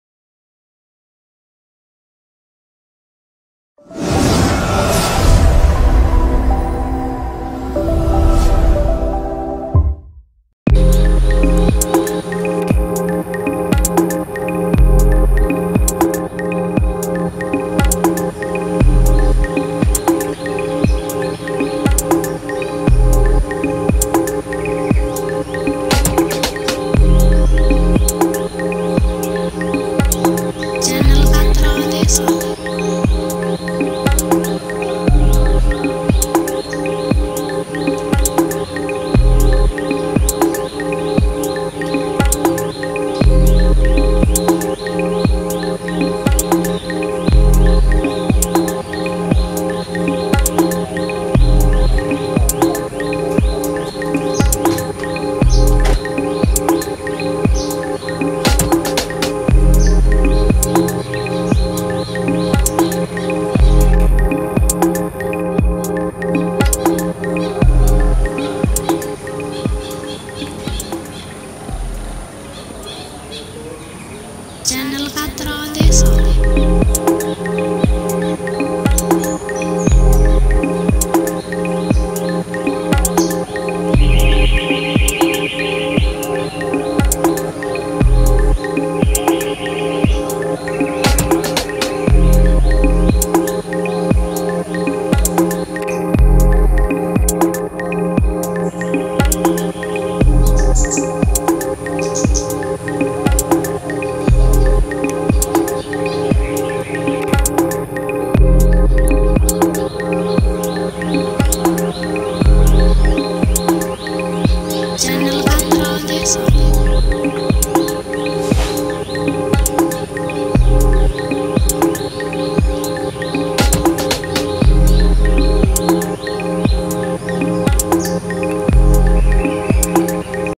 Tanaman Brotowali
Channel Katro Ndeso menayangkan video bentuk tanaman brotowali sebagai salah satu tanaman obat herbal yang banyak khasiatnya.  Bratawali atau brotowali atau dikenal dengan nama ilmiah Tinospora cordifolia merupakan tanaman obat tradisional yang mudah ditanam di pekarangan atau kebun. Manfaat bratawali atau khasiat brotowali bisa digunakan dengan olahan rebusan batang, rendaman kulit yang sudah dikeringkan, dan juga akarnya

Rasa brotowali (Heart-leaved moonseed) memang cukup pahit, namun memang cukup banyak khasiatnya untuk menyembuhkan gangguan kesehatan atau penyakit. Tentu saja dengan olahan dan takaran yang benar untuk bisa memanfaatkan untuk obat. Kandungan kimia dalam brotowali seperti alkaloid, berberina, kolumbina, palmatin, kolumbin, kokulin, glikosida pikroretosid, pikroretin, hars, berberin, dll. Khasiat bratawali untuk obat rematik, mengurangi gula darah, menurunkan panas, dan membantu mengurangi gejala kencing manis.